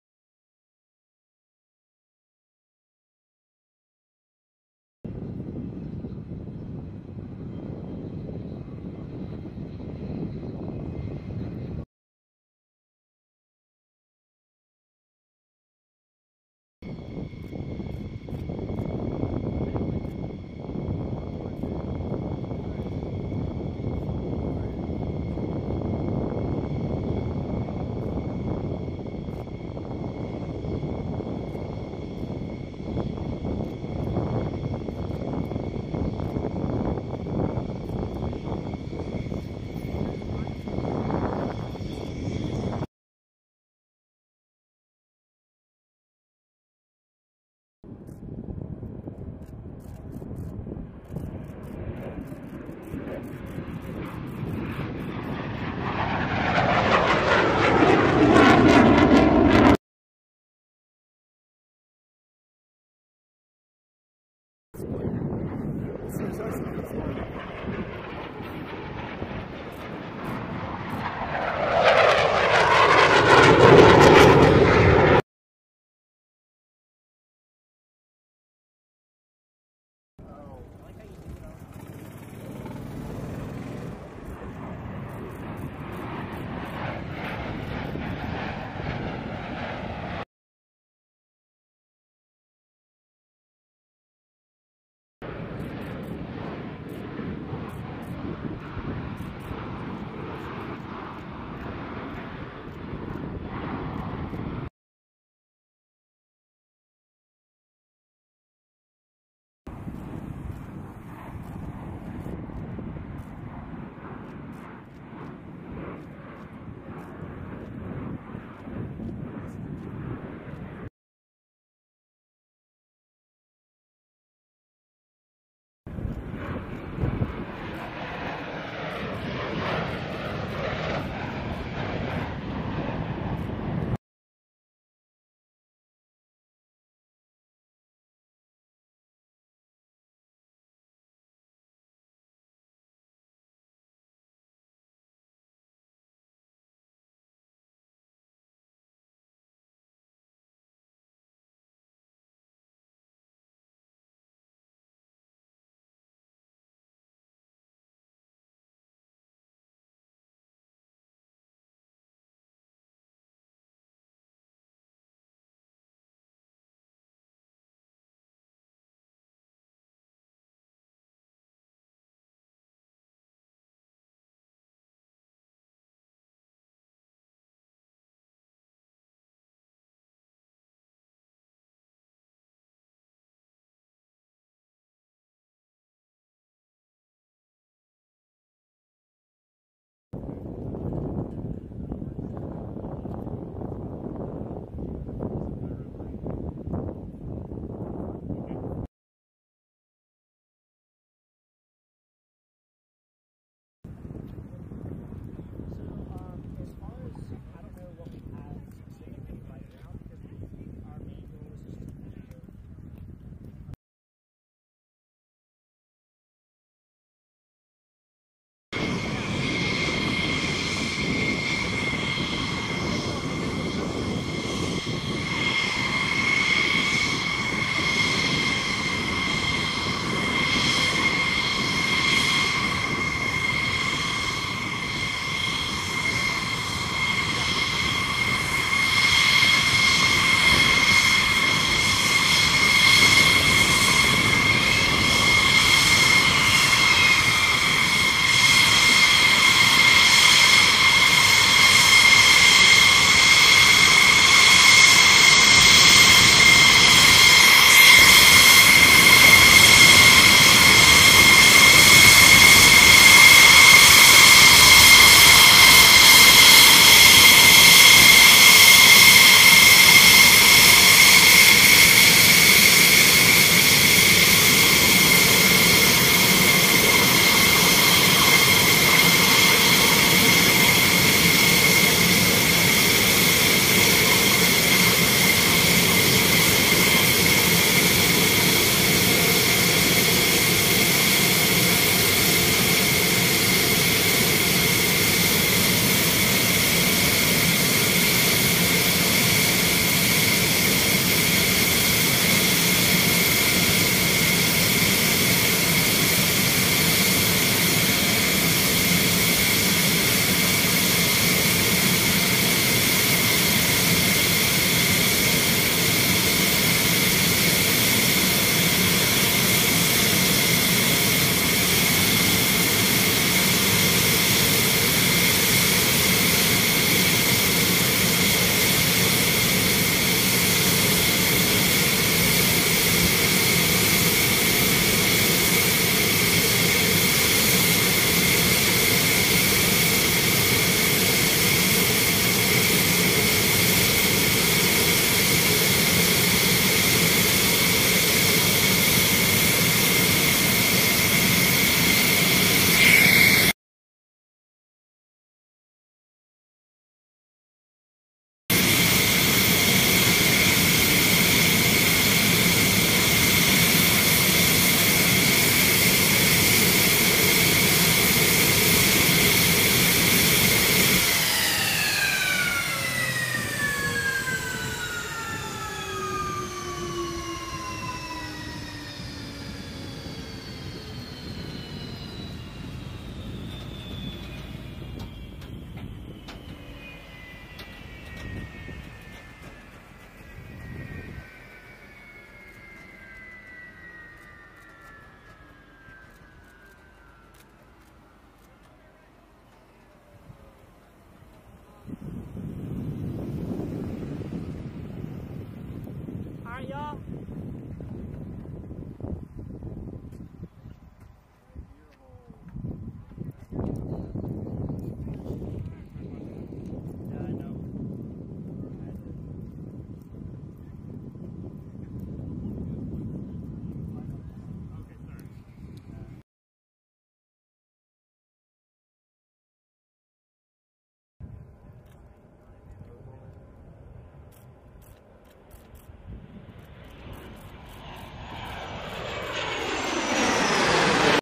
Thunderbirds arrival to Joint Base Andrews for the Joint Base Andrews 2022 Air & Space Expo.
Broll footage of the Thunderbirds arrival to Joint Base Andrews for the Joint Base Andrews 2022 Air & Space Expo.

Thunderbirds Arrival Broll Stringer
UNITED STATES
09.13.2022

Film Credits: Video by Airman Essence Myricks
316th Wing Public Affairs